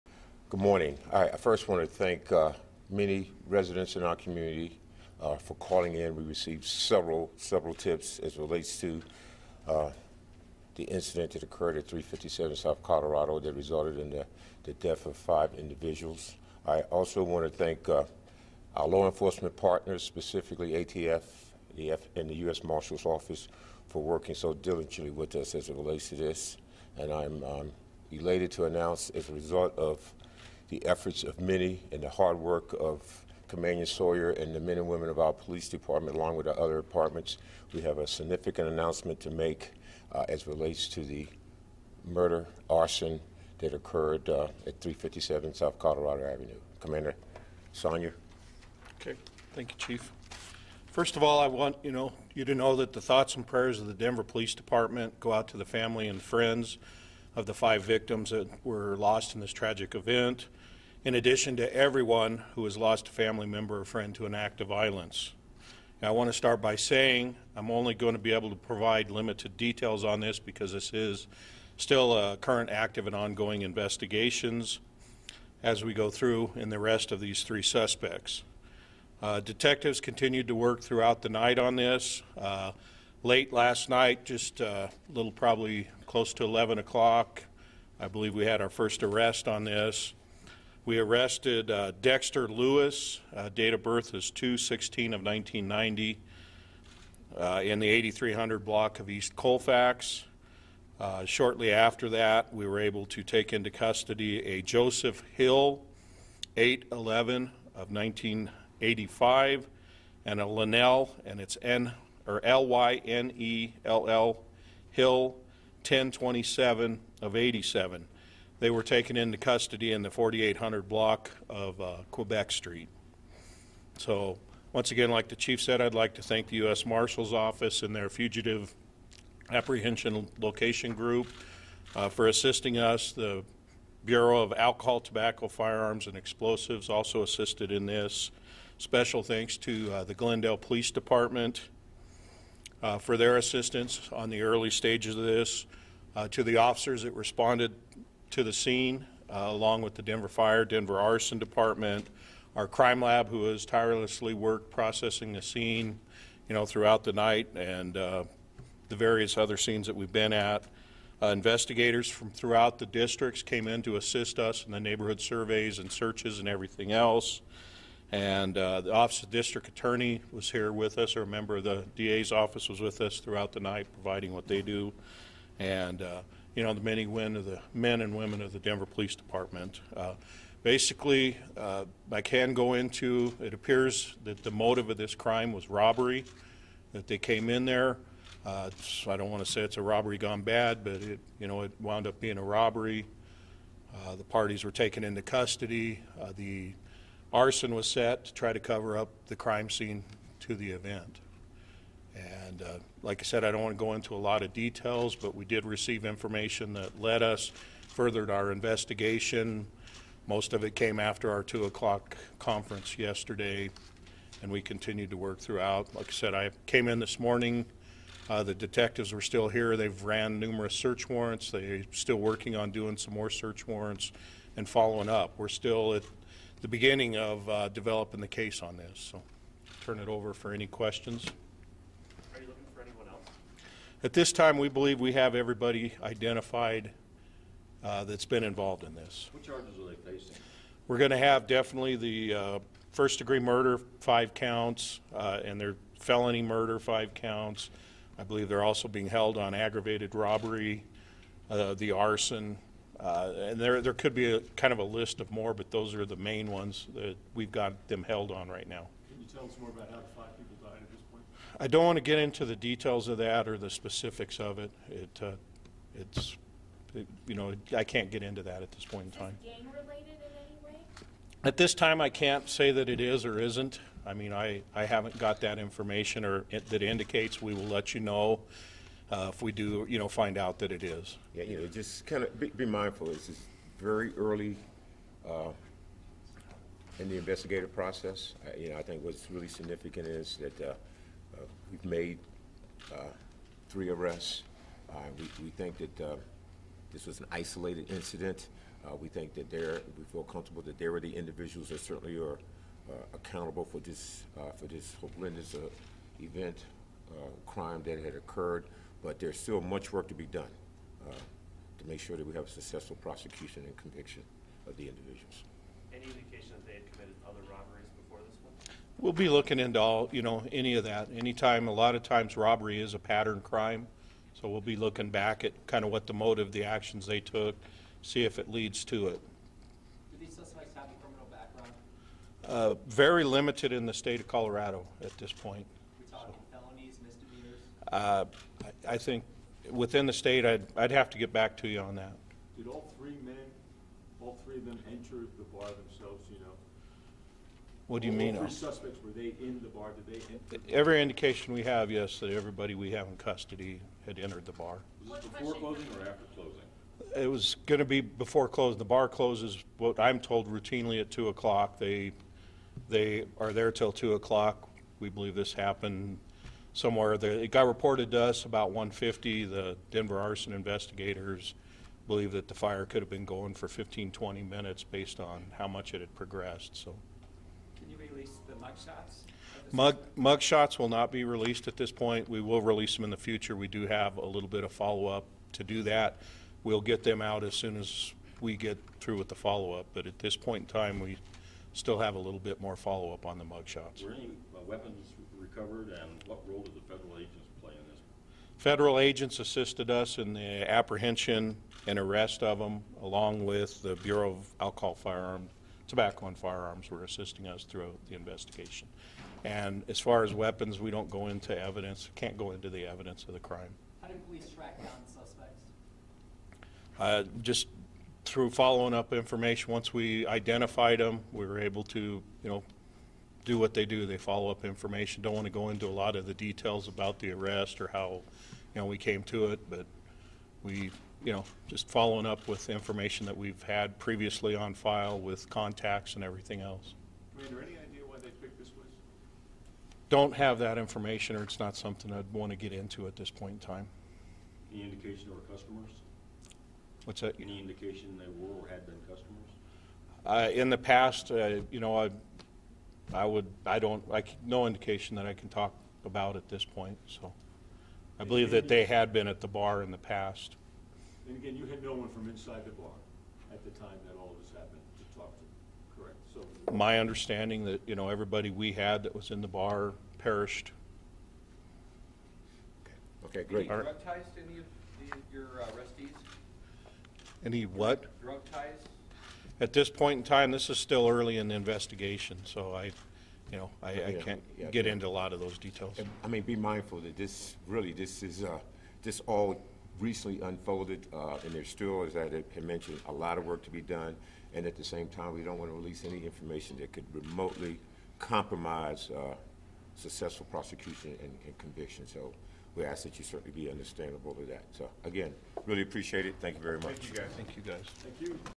RAW VIDEO: Denver Police talk about homicide arrests
10/17/12 6:30am: Denver Police news conference on the arrests of 3 suspects in the fire and homicides at Fero's Bar and Grill.